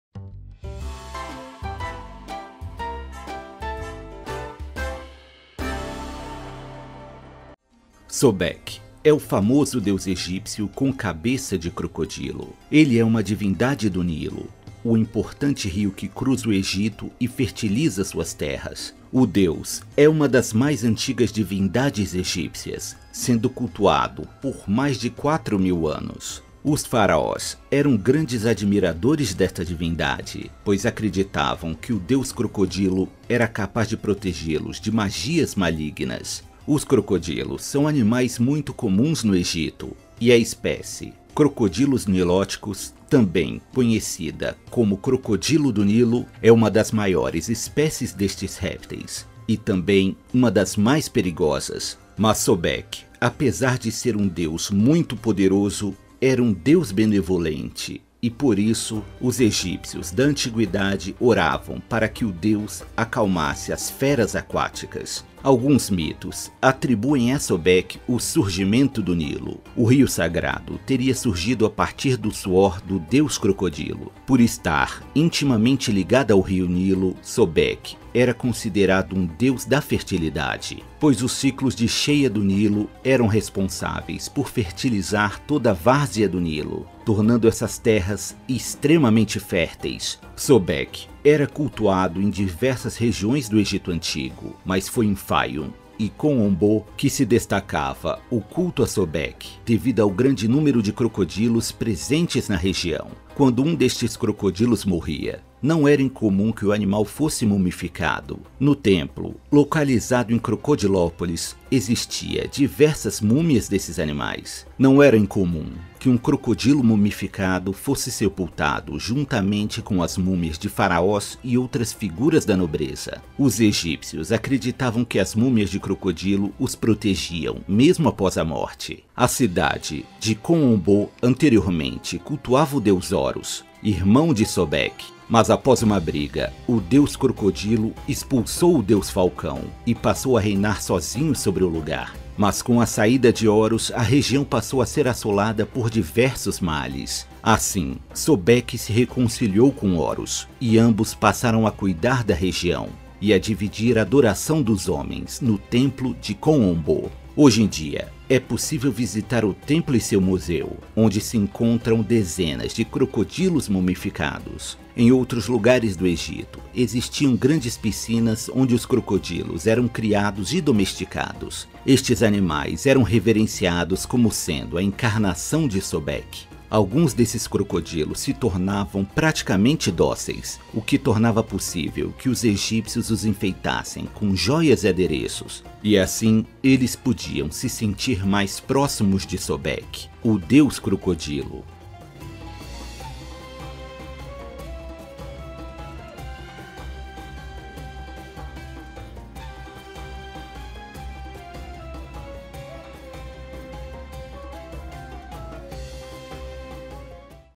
Sobek: O Poderoso Deus Crocodilo da Mitologia Egípcia - Dicionário Mitológico - Foca na História
Mitologia Egípcia - Sobek: O Poderoso Deus Crocodilo

Wiki: Sobek é uma divindade do Antigo Egito, sendo representado como um crocodilo (colocado num altar ou santuário) ou então como um homem com cabeça de crocodilo, muitas vezes, neste caso, ostentando uma coroa com duas grandes plumas, o disco solar e uma ou mais ureus (serpentes sagradas).

- William Jimbo
- Enderson Hanzen
- Ronald Verri
- e outros 3 mamíferos que preferiram continuas anónimos